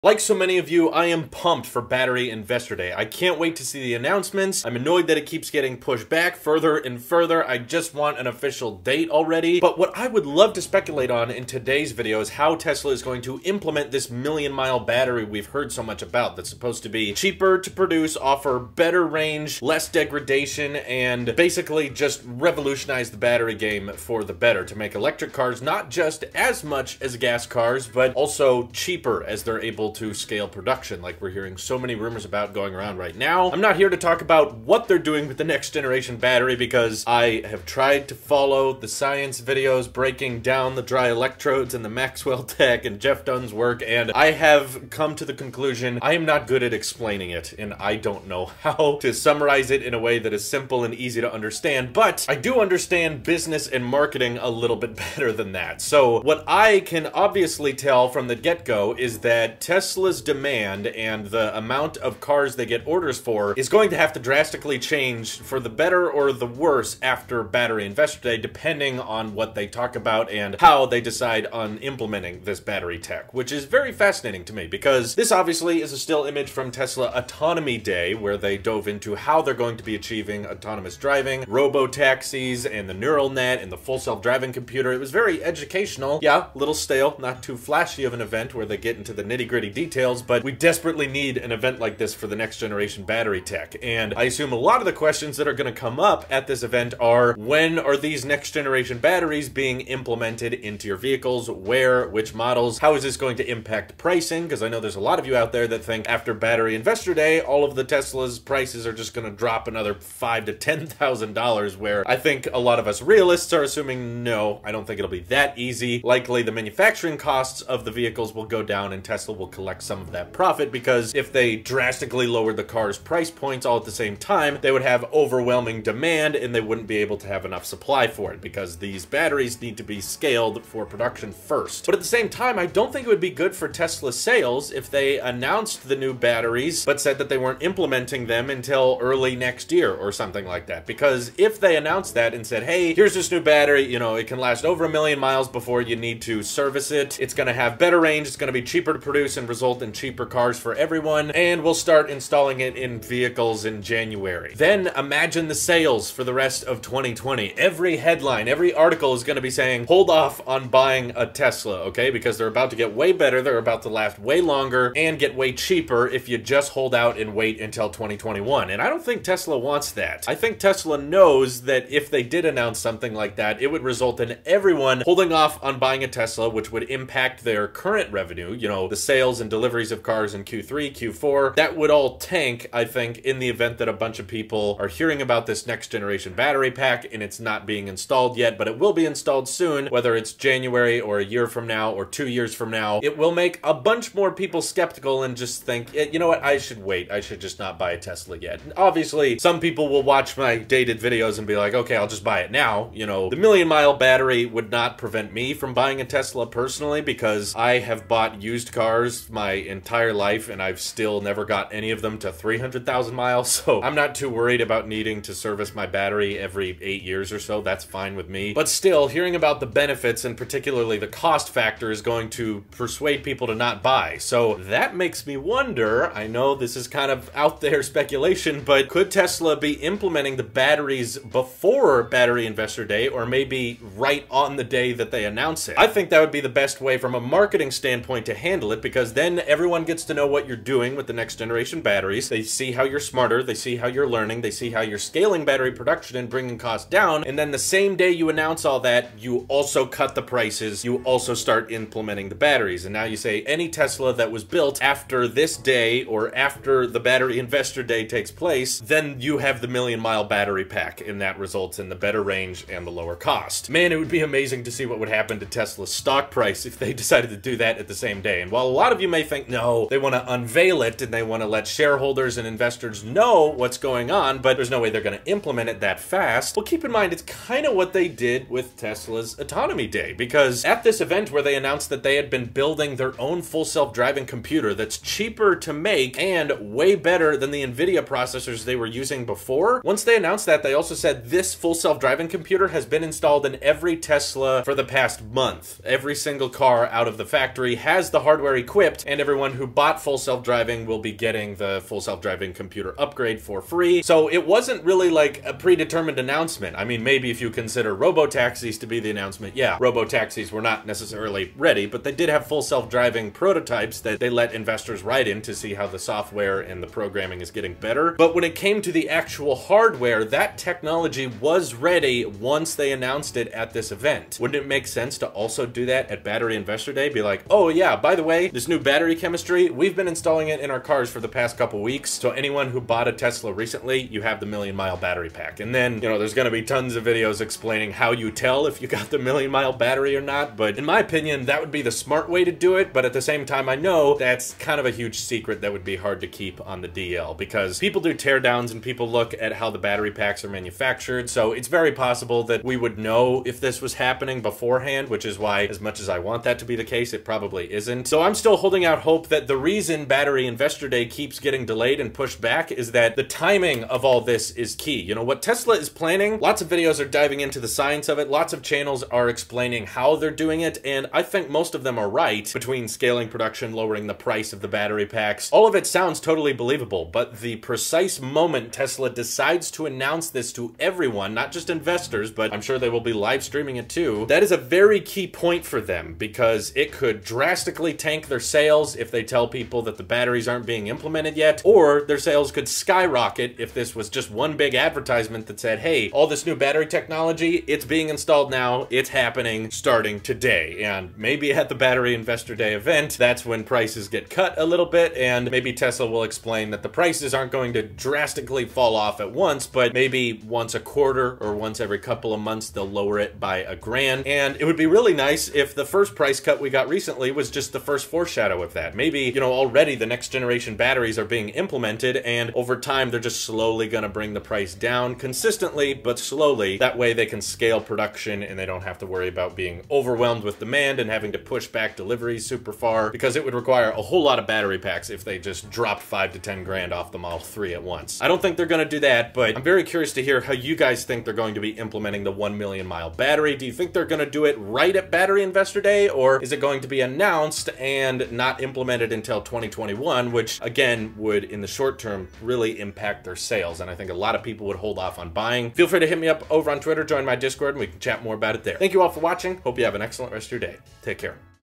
How will Tesla Implement the Million Mile Battery?
When do you think Tesla's will be rocking the next gen Battery?